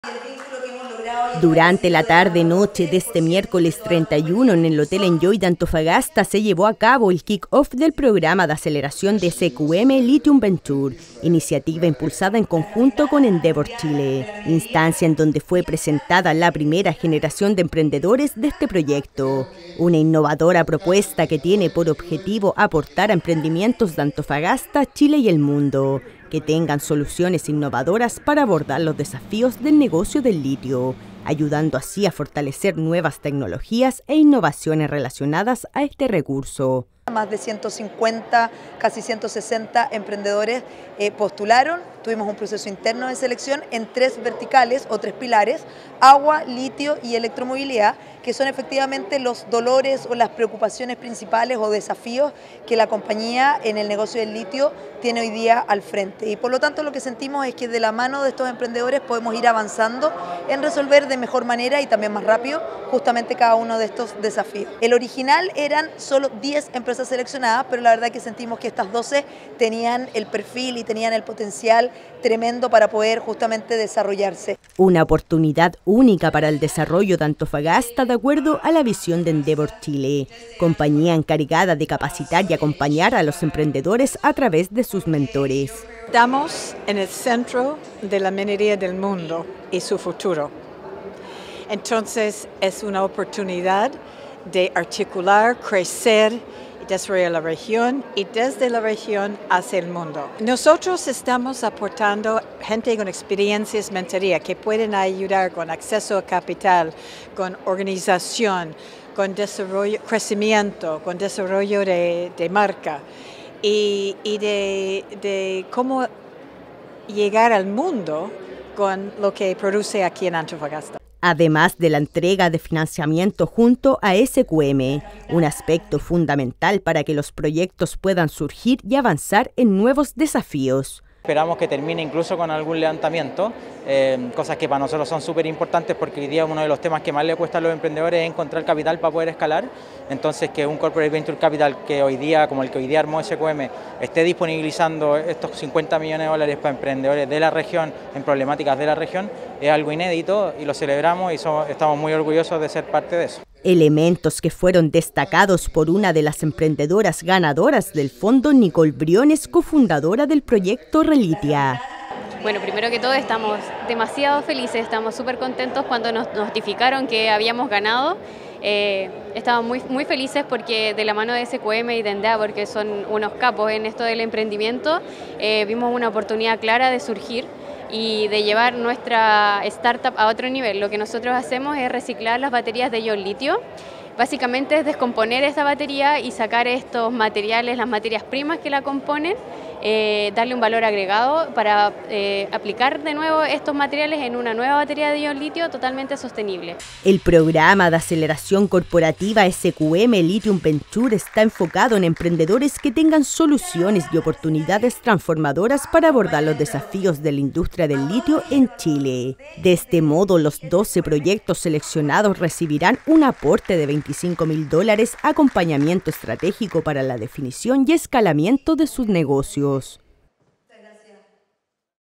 12 emprendedores seleccionados en el Programa de Aceleración de SQM Lithium Ventures
Durante la tarde noche de este miércoles 31, en el Hotel Enjoy de Antofagasta, se llevó a cabo el Kick off del Programa de Aceleración de SQM Lithium Ventures, iniciativa impulsada en conjunto con Endeavor Chile…. Instancia en donde fue presentada la primera generación de emprendedores de este proyecto.

Una innovadora propuesta que tiene por objetivo aportar a emprendimientos de Antofagasta, Chile y el mundo que tengan soluciones innovadoras para abordar los desafíos del negocio del litio; ayudando así a fortalecer nuevas tecnologías e innovaciones relacionadas a este recurso….